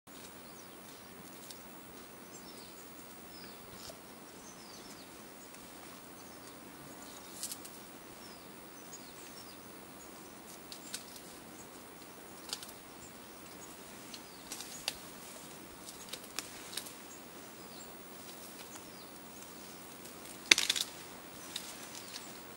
Two Infant Friends Play | Dian Fossey Gorilla Fund
Gorillas begin forging friendships at very young ages. 💛

In this video from the Fossey Fund vault, watch infants Kabeho and Mubyeyi play together. These infants were born one month apart, and they live in Kureba's group.

📹  Video: Cedric Ujeneza

Our videos should not be reposted on YouTube without permission.